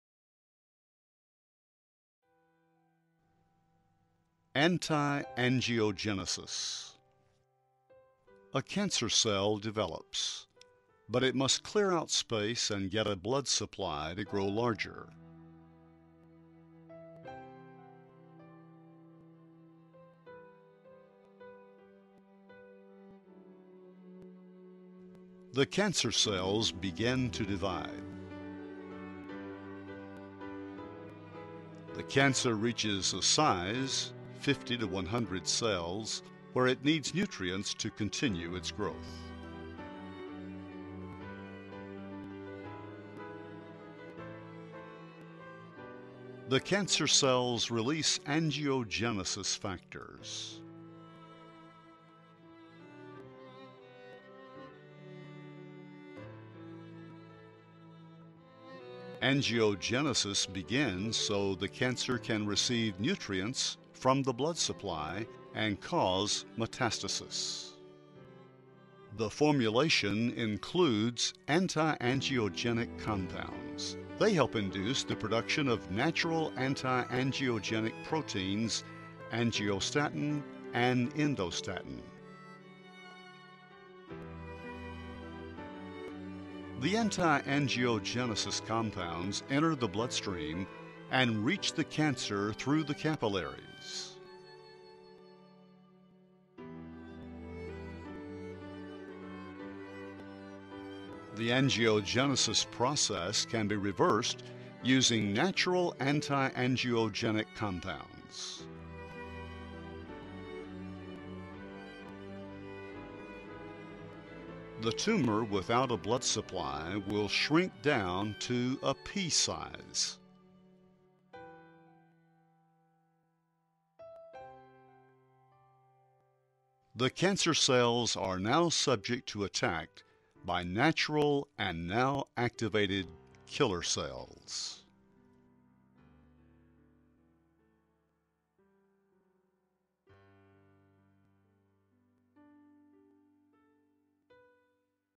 Anti-Angiogenesis - Antiangiogenesis
Cancer cells can release angiogenesis factors, which cause capillaries to grow from blood vessels and receive nutrients from the blood supply, which in turn allows the tumor to grow and cause metastasis. Antiangiogenic compounds can reverse the effects of angiogenesis. This happens by cutting off the blood supply to the tumor, which will cause it to shrink down to a size where the body's immune system can kill off the rest of the cancer.